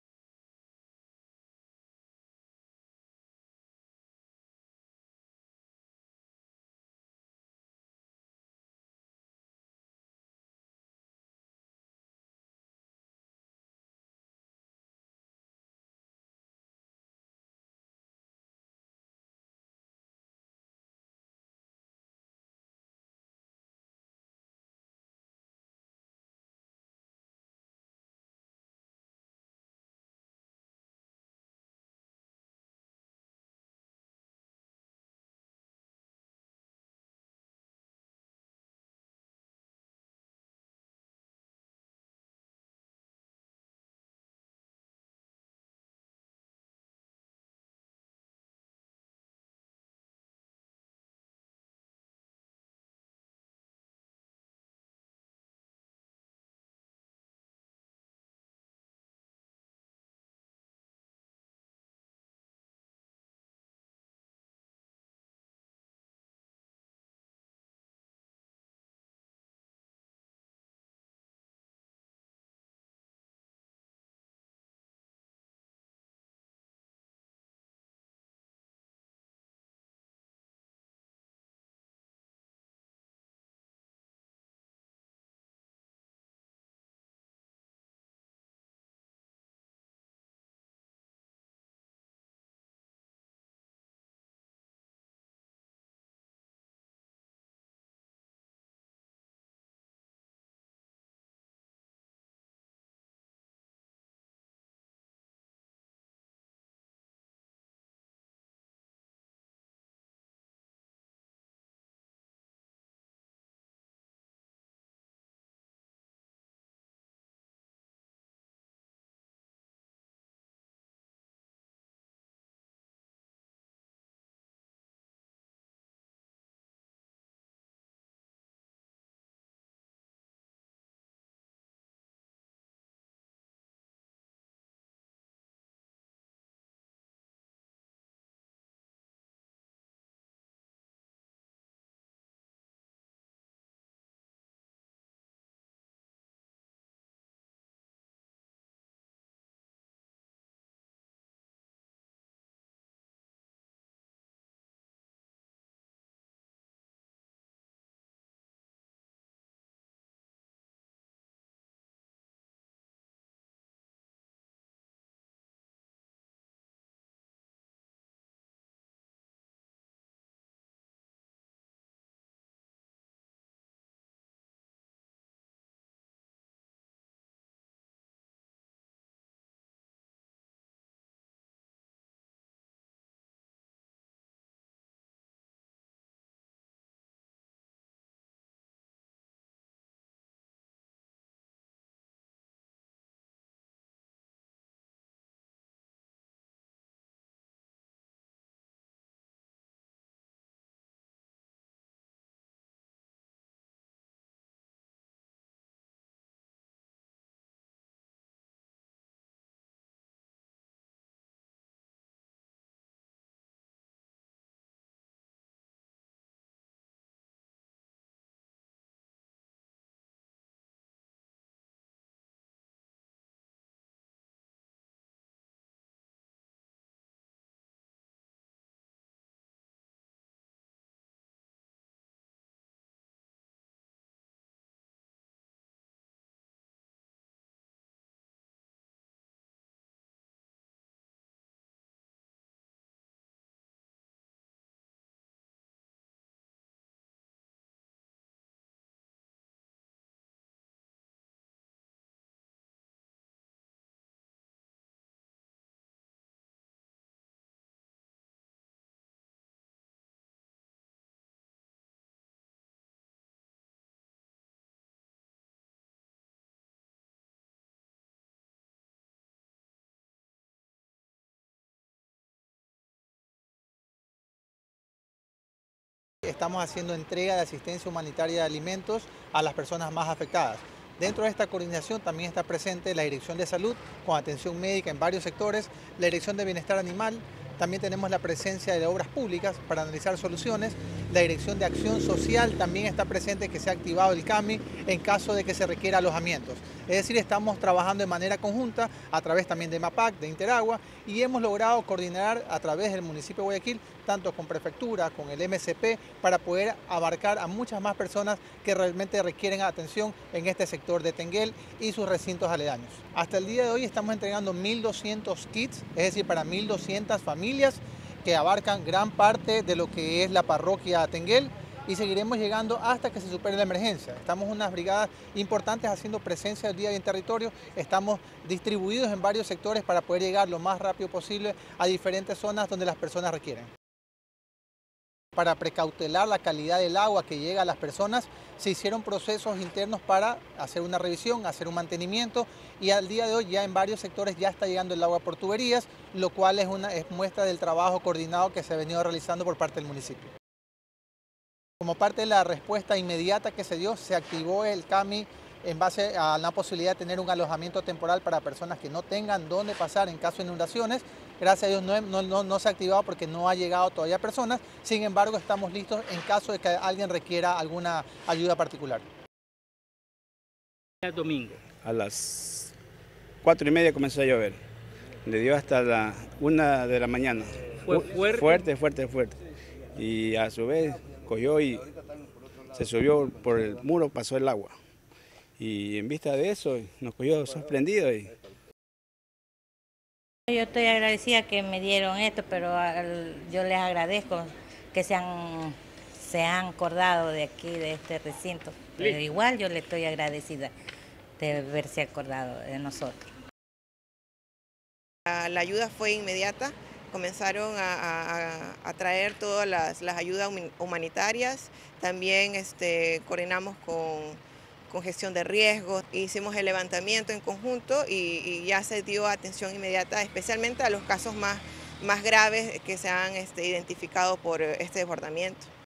Direcciones municipales y Segura EP entregan ayuda a los habitantes de Tenguel afectados por lluvias
El Municipio de Guayaquil y sus entidades vinculadas distribuyeron ayuda humanitaria a 1.200 familias de la parroquia Tenguel, afectadas por las lluvias de los últimos días. Kits de alimentos, atención médica, entrega de medicina y evaluación de mascotas fueron los servicios que ofrecieron las direcciones municipales y Segura EP.

Las lluvias, unido a la subida de la marea, ocasionó el desbordamiento del río Gala que rompió un muro de contención. Eso provocó la inundación del recinto San Rafael. Otros barrios afectados fueron Virgen del Cisne, Diez de Agosto, Palo Prieto, 12 de Julio, 9 de Octubre, La Floresta, Los Ciruelos y el recinto Puerto El Conchero.

“Hasta hoy estamos entregando 1.200 kits de alimentos, es decir para 1.200 familias, que abarcan gran parte de la parroquia y seguiremos llegando hasta superar la emergencia”, aseguró Álex Anchundia, gerente de Gestión de Riesgos de Segura EP.

Las brigadas médicas de la Dirección de Salud se desplegaron por los diferentes sectores afectados evaluando el estado de los moradores, a la vez que se les entregó medicinas y vitaminas. Similar trabajo realizó la Dirección de Bienestar Animal a las mascotas.

Respecto del agua potable, Anchundia comentó que técnicos de EMAPAG llegaron al sitio para constatar la normalización del servicio, desde las 23:25 de este lunes 12 de febrero de 2024.

“Se suspendió el servicio para precautelar la calidad del agua, pero luego del mantenimiento y un proceso interno se restableció casi de inmediato y ya hay agua por tubería”.

De su parte DASE EP activó de inmediato un albergue temporal en el CAMI con veinte espacios en caso de que algunas familias lo requieran. “No han llegado personas, pero el albergue está listo en caso de que alguien lo requiera”, agregó Anchundia.

La Dirección de Obras Públicas se unió al contingente para evaluar las afectaciones, sobre todo en el caso del muro que cedió por la fuerza del río.

Habitantes de las zonas afectadas se sintieron aliviados por la ayuda humanitaria. Jaime Zumba, del recinto San Rafael, recordó que a las 16:30 del domingo comenzó a llover y duró hasta la madrugada. “El río se salió y traspasó el muro, nos sorprendió el agua, necesitamos la ayuda”.

Julia Solórzano, del sector Puerto El Conchero, tras recibir el kit de alimentos también comentó que “estoy agradecida con el Municipio que me dieron esta ayuda”.

De su parte Liliana Símbala, presidenta del GAD parroquial de Tenguel, manifestó que desde que se registró la alerta se informó a las instituciones, principalmente al Municipio, gracias a la coordinación comenzaron a traer todas las ayudas humanitarias. “Con Gestión de Riesgo se hizo el levantamiento de información para la atención inmediata”, destacó.